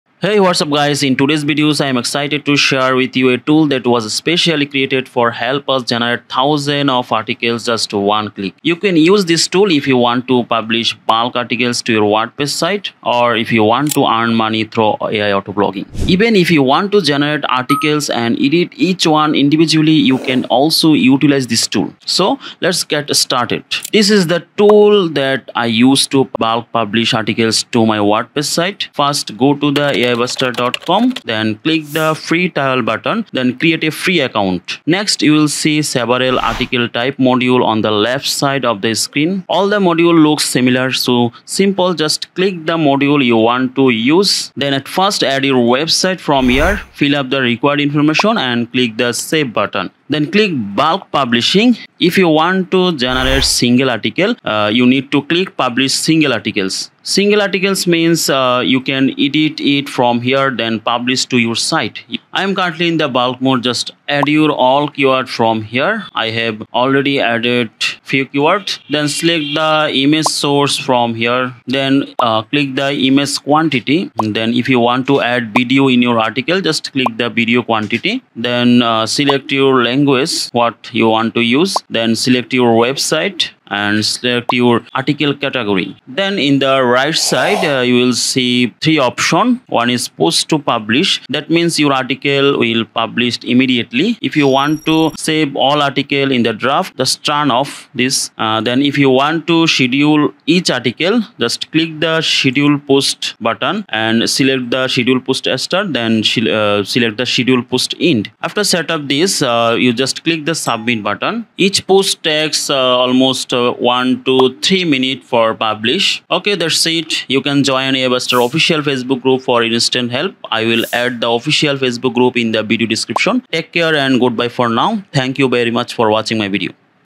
How To Generate 13000 of Article Just Few Click? | Auto Blogging Tools | Best AI Writing Tools
AI Buster is an exceptional AI-powered content generator for auto blogging and is widely regarded as one of the best AI writing tools of 2023.

⏰ Key-frames⏰

00:00 Introduction
00:22 AI Buster - Bulk Article Generator Tools
00:40 Choose Module & Add Website
00:50 Place Keyword & Generate Article
01:26 How To Publish/Schedule/Draft Articles
01:56 Support Team